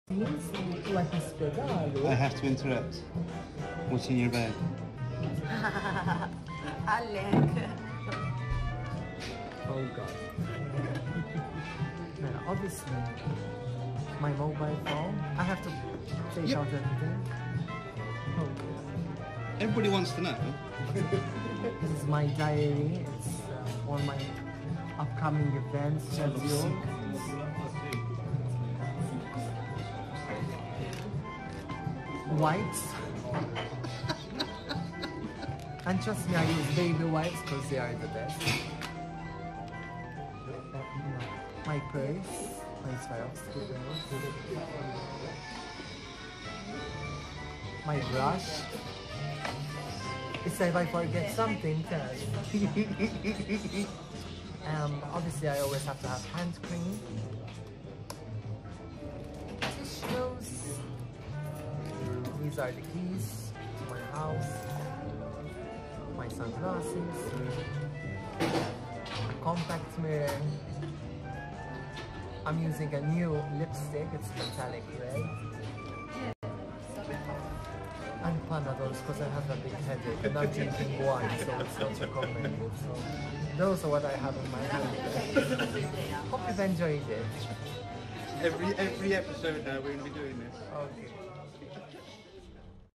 The Clintess - What's In Your Bag?
Everyone wants to know what The Clintess has in her bag. Check out this first video during filming of her reality show "A Day With The Clintess", produced by MT Lifestyle & Travel Vlog.